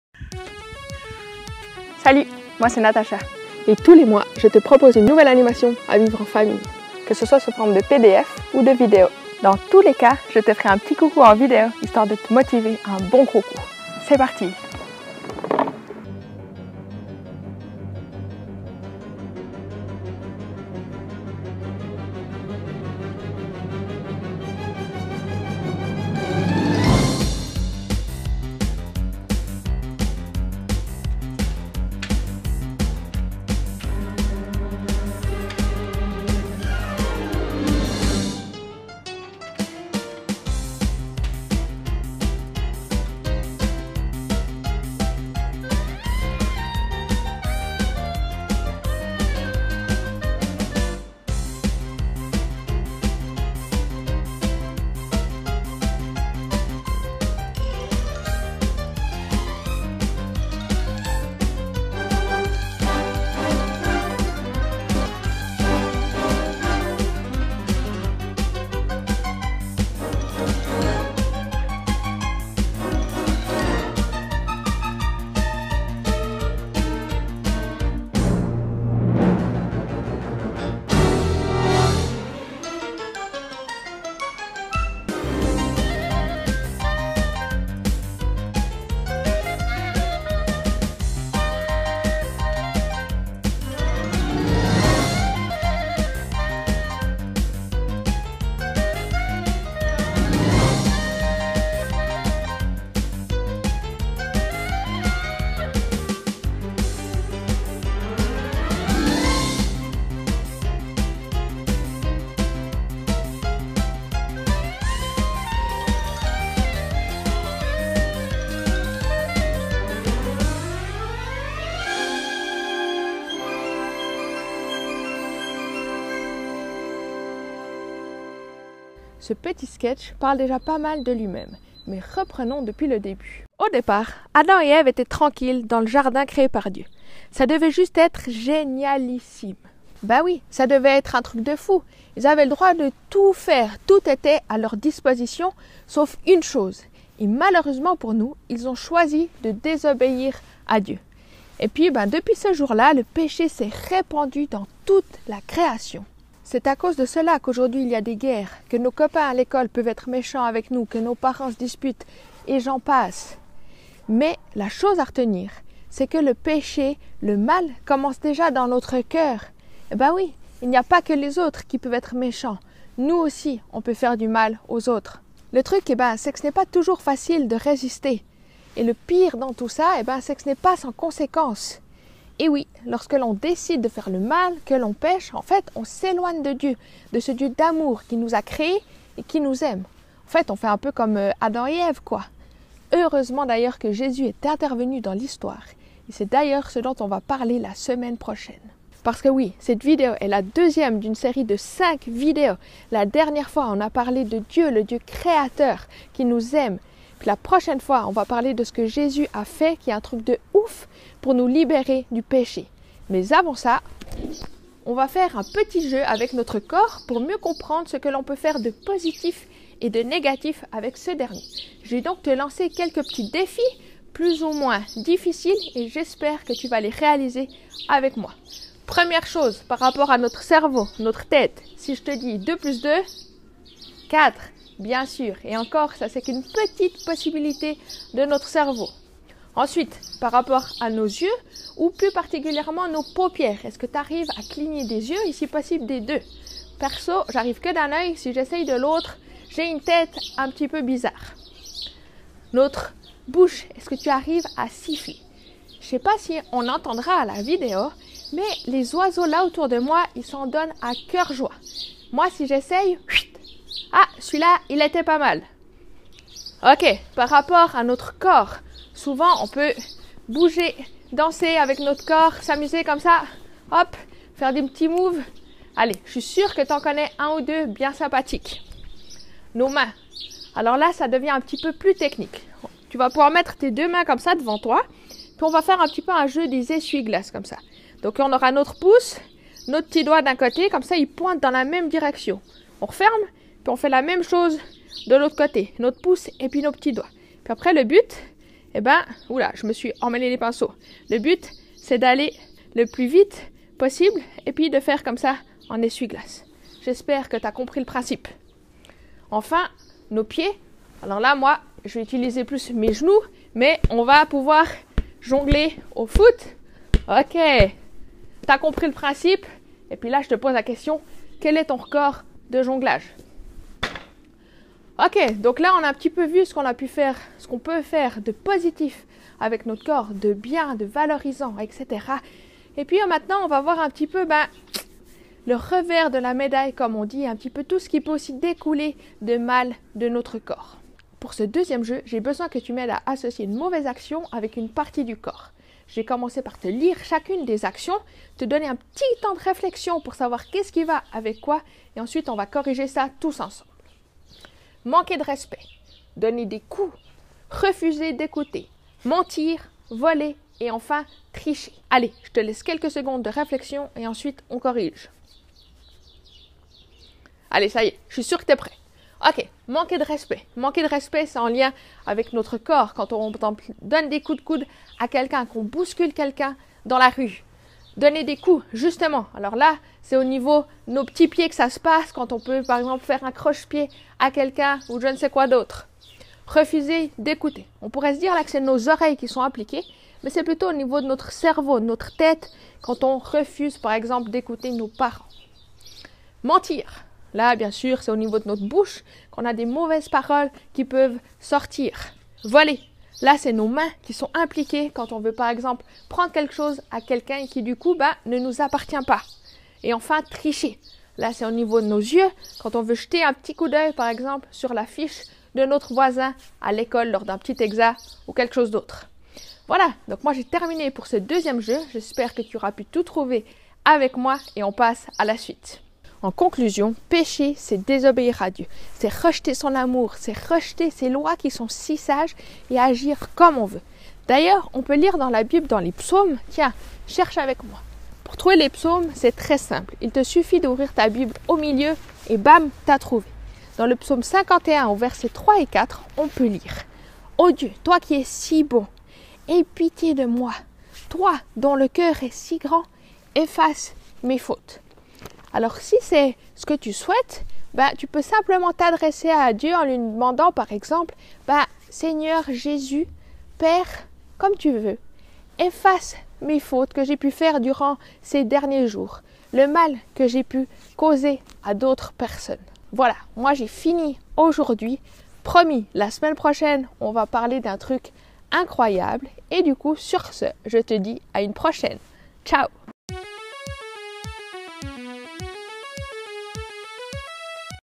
Péché : ne pas toucher ! (épisode 2/5)
L'évangile expliqué aux enfants en 5 étapes !